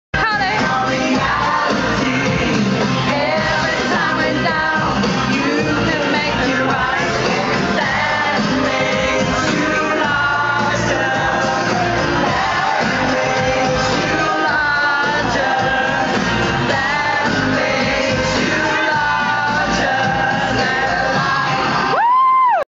Daytime drinking in Viaduct
Carly singing and dancing at 1pm in a drunken shambles hahaha! Gotta love our daytime drinking seshes!